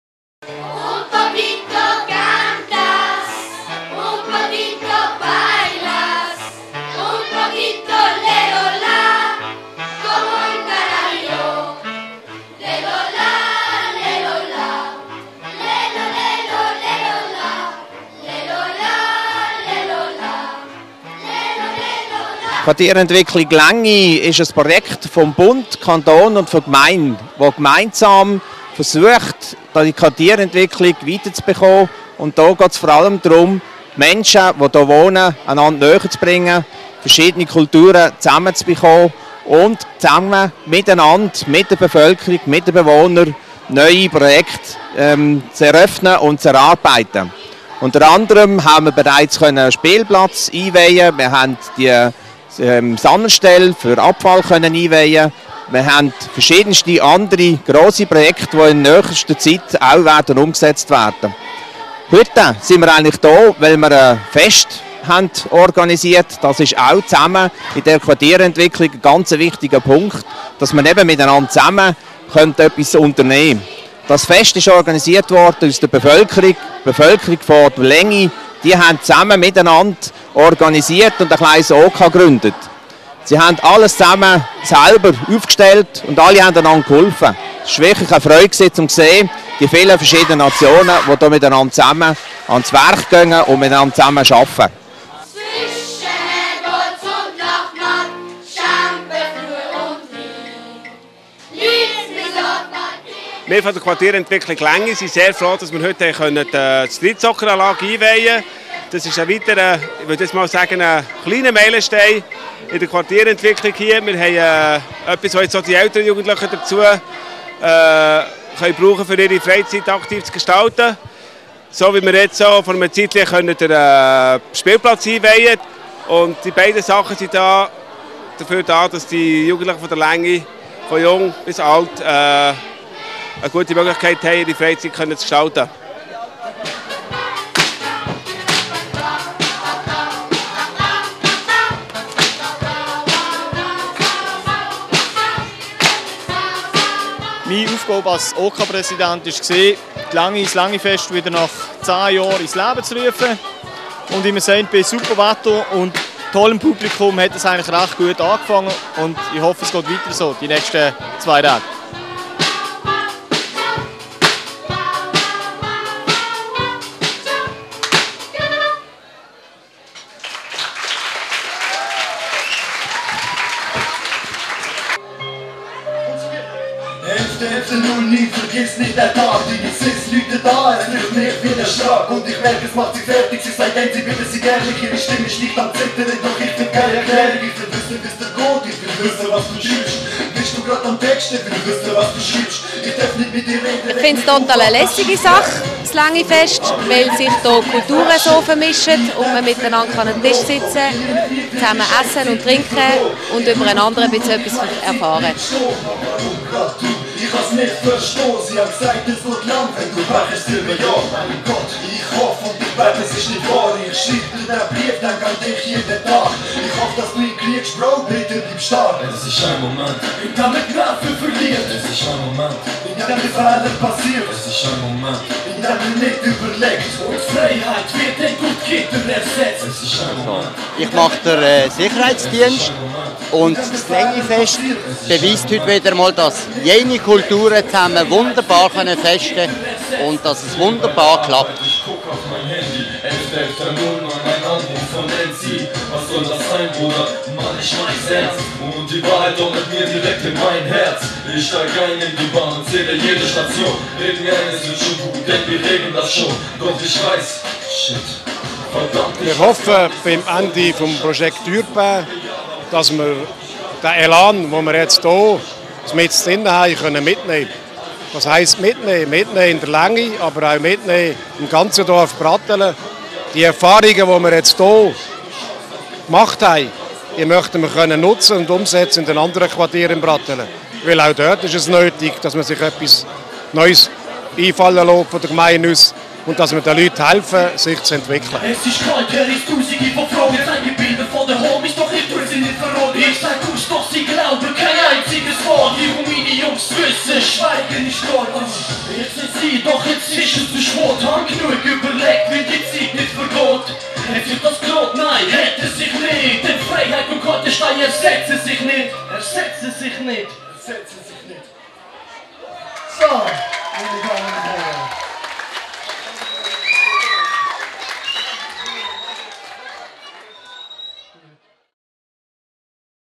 Quartierentwicklung Pratteln Längi 2010
Resultate werden sichtbar. Im Rahmen der "Quartierentwicklung Pratteln Längi" wurde am Wochenende vom 4./5. September 2010 ein Quartierfest durchgeführt - erstmals wieder seit etwa 10 Jahren. Bei dieser Gelegenheit wurden die neue Street-Soccer-Anlage und die Unterflur-Wertstoffsammelstelle eingeweiht.  Im Beitrag werden die Interviewpartner vom Längichor und den Prattler Rappern begleitet.